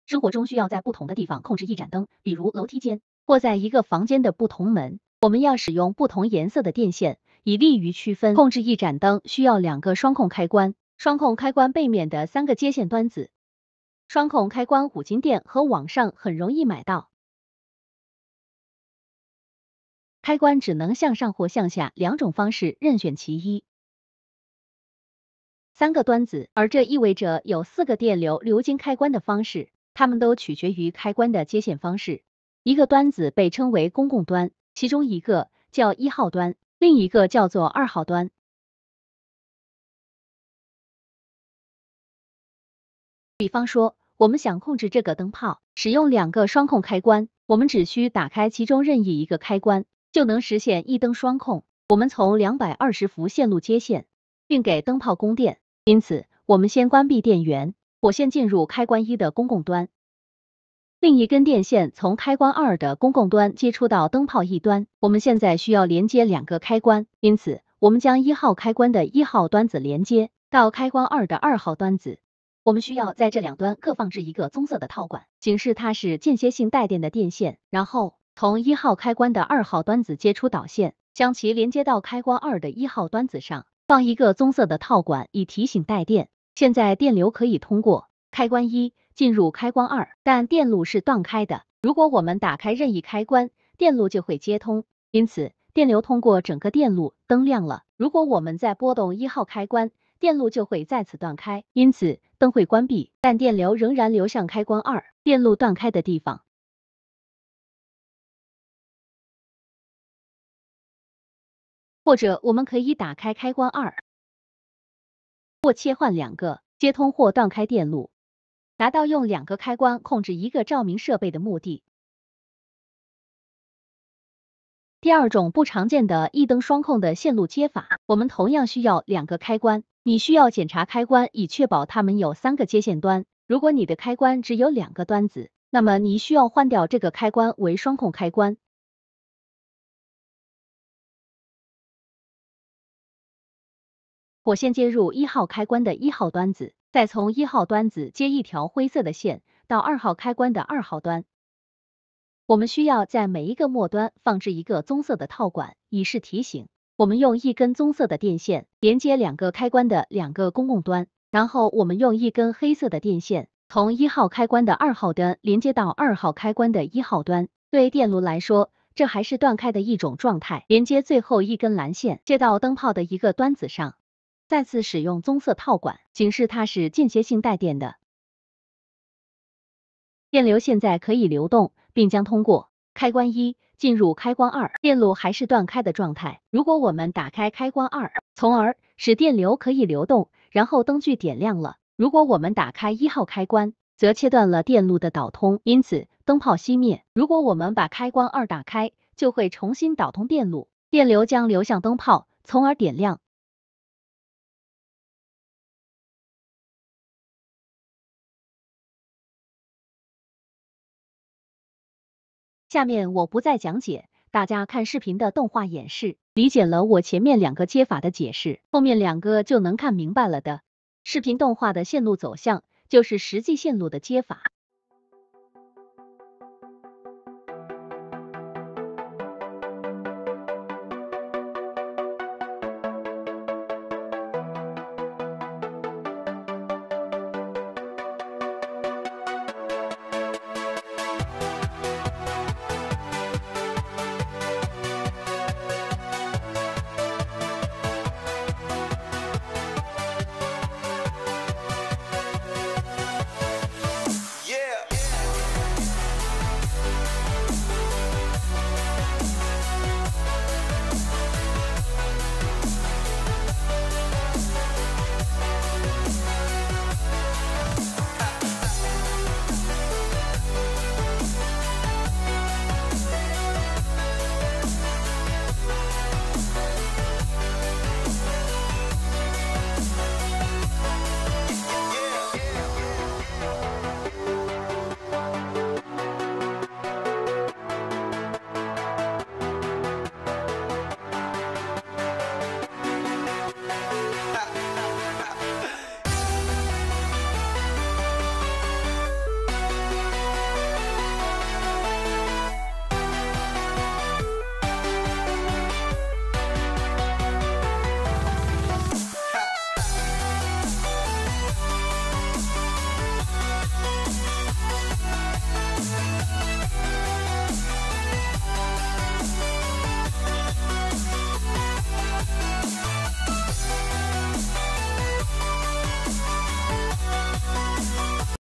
一灯双控的4种接法，我猜就算老电工，也只知其一
A light double control of the 4 kinds of connection, I guess even the old electrician, but also know only one of